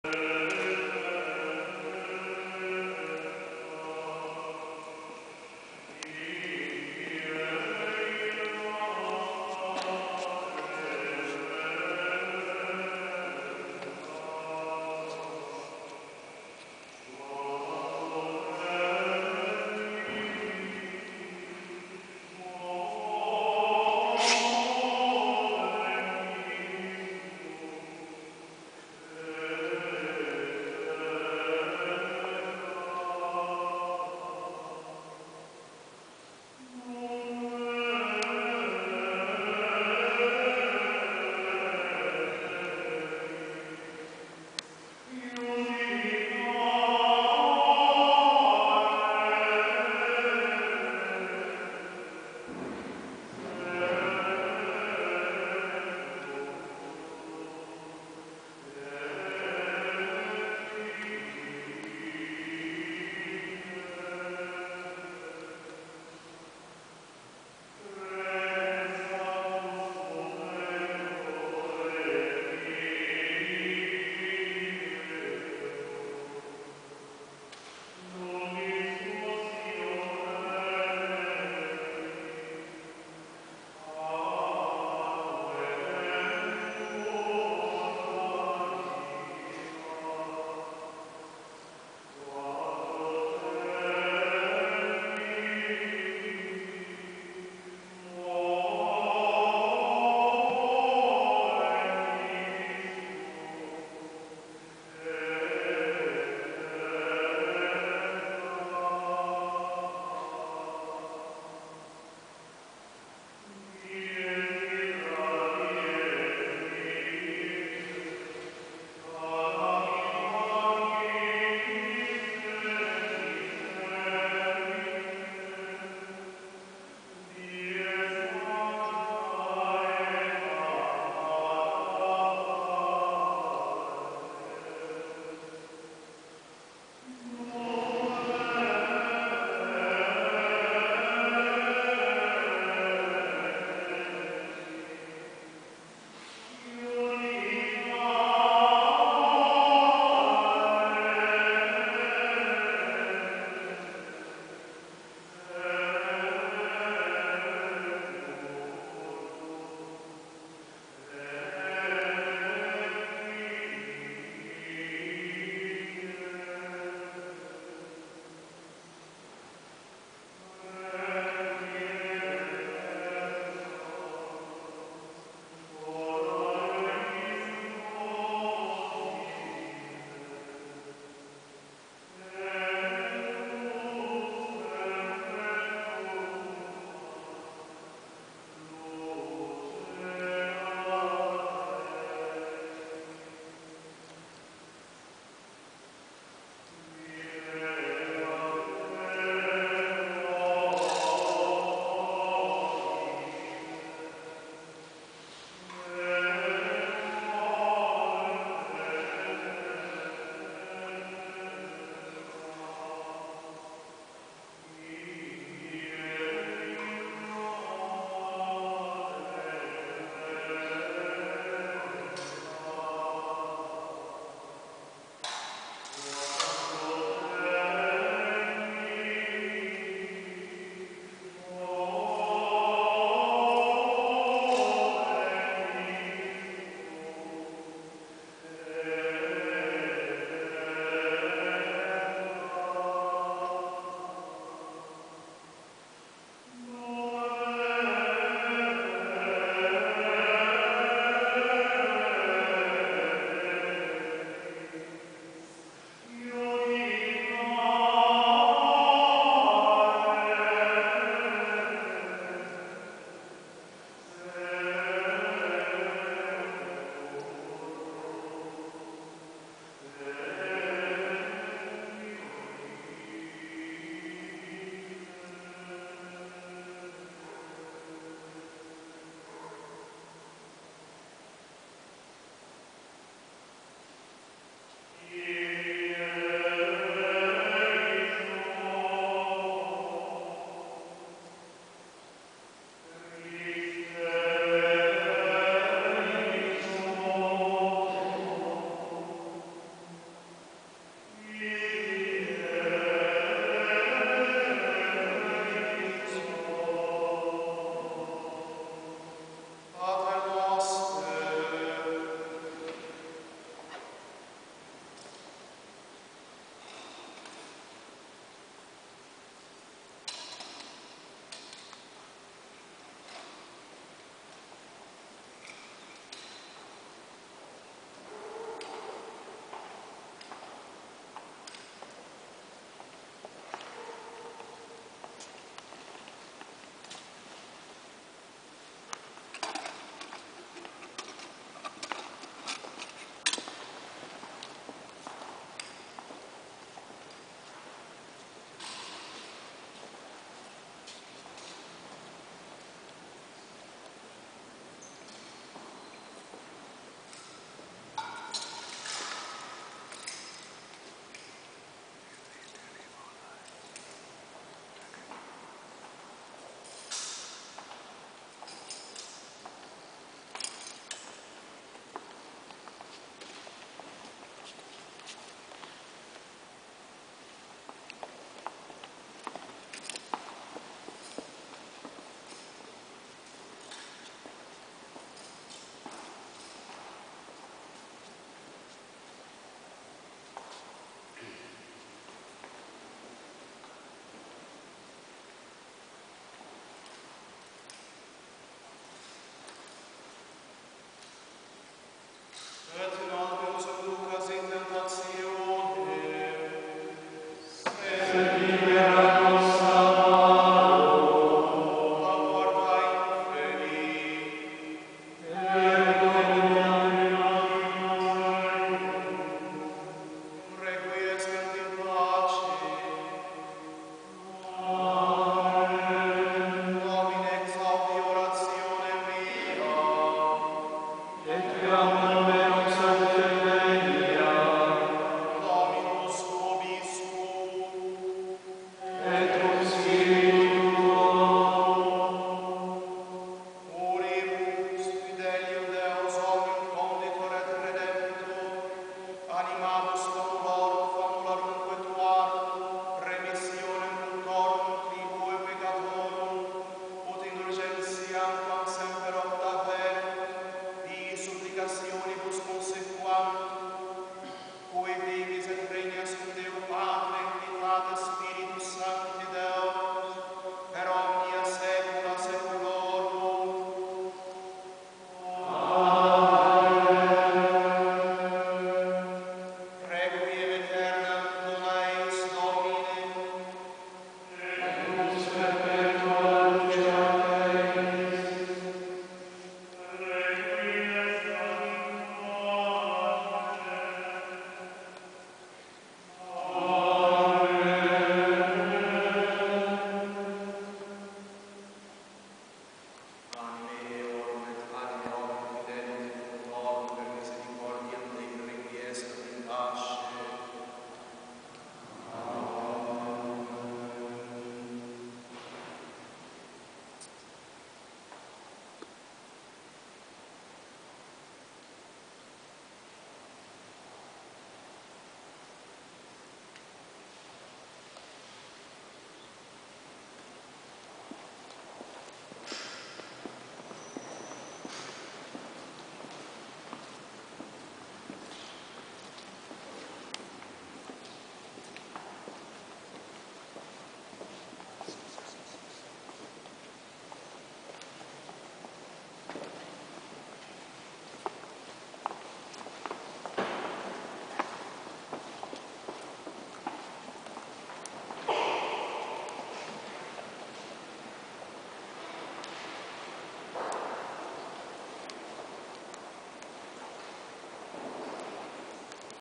FSSP Rome Requiem Mass
November, 2008: All Souls day in Rome with absolution at the catafalque.  Church of the Ss. Trinita dei Pellegrini.